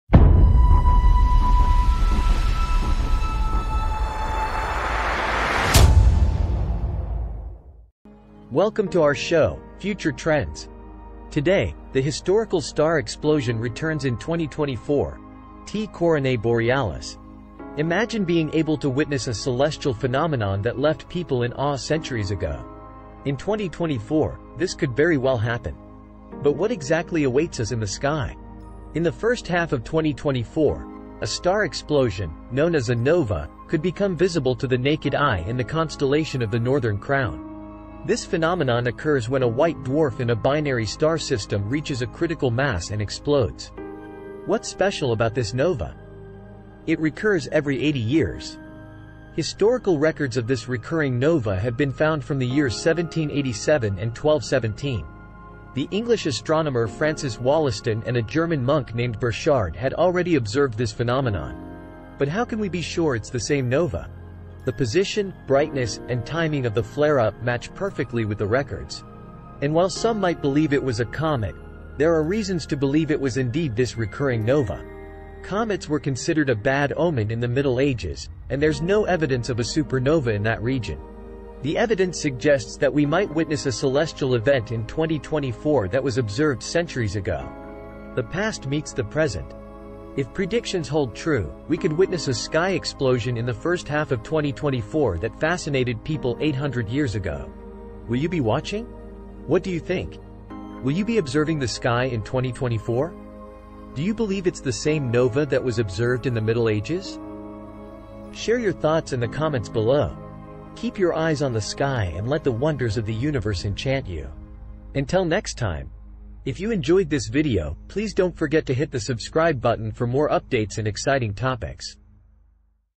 The Mystery Star Explosion Returns in 2024: T Coronae Borealis
In 2024, a recurring celestial event, a Nova, is expected to be visible in the Northern Crown constellation. This Nova, which occurs when a White Dwarf in a binary star system explodes, has a unique 80-year cycle. Historical records indicate observations of this event from as far back as 1787 and 1217. While some believe it might have been a comet, evidence suggests it's the recurring Nova. This celestial phenomenon, observed centuries ago, will once again grace our skies in 2024, bridging the past and the present. Observers are encouraged to witness this rare event and share their thoughts.